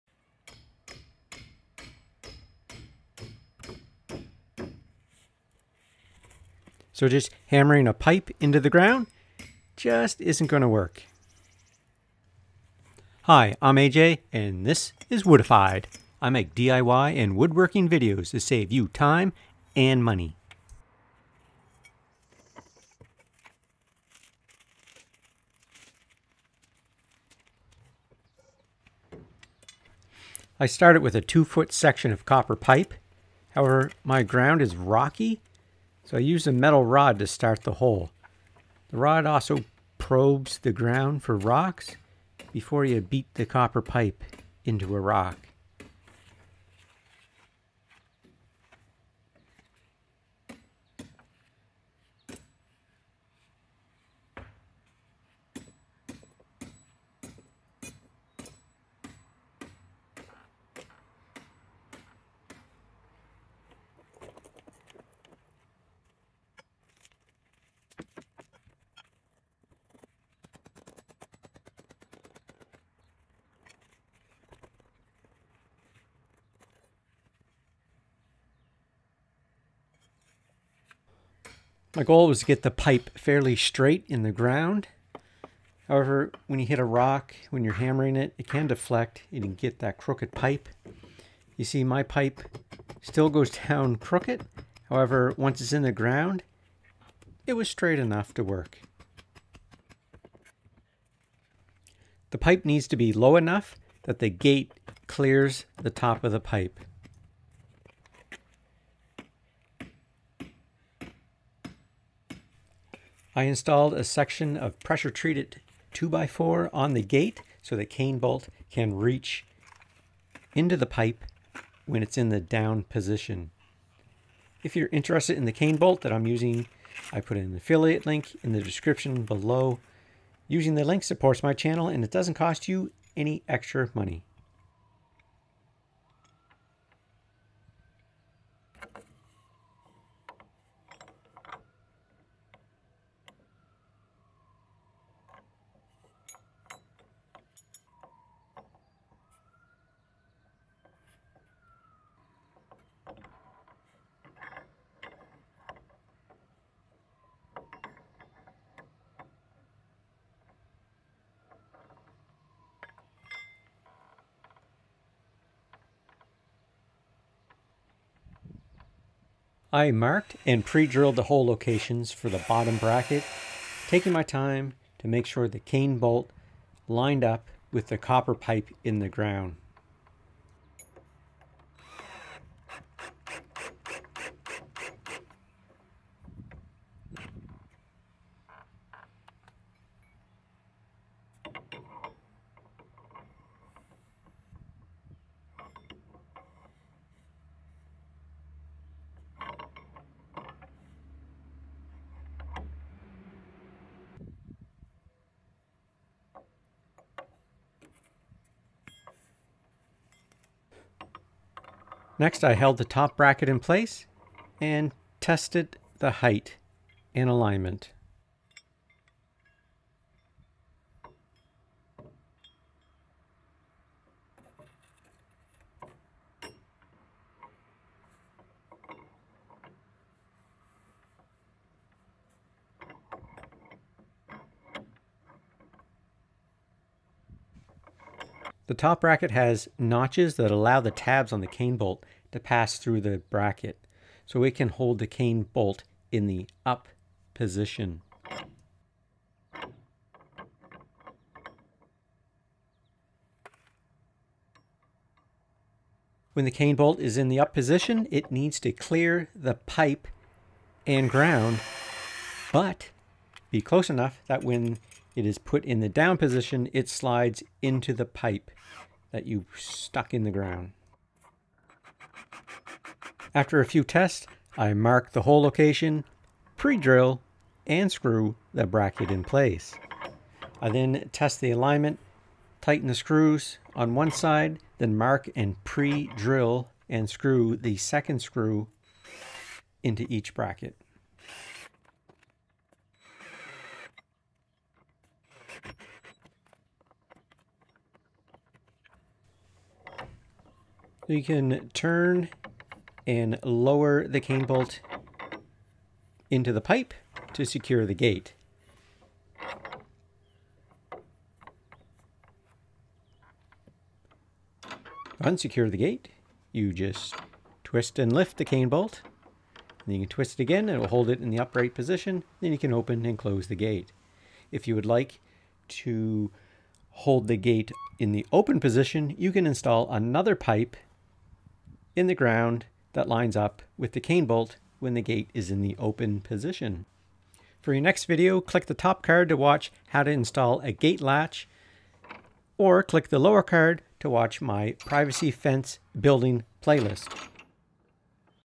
How To Secure A Double Wooden Gate using a cane bolt
Secure your double gate using a cane bolt.  I installed a double wooden gate on my 6 foot privacy fence.  This video shows you how to install a cane bolt on a wooden fence gate.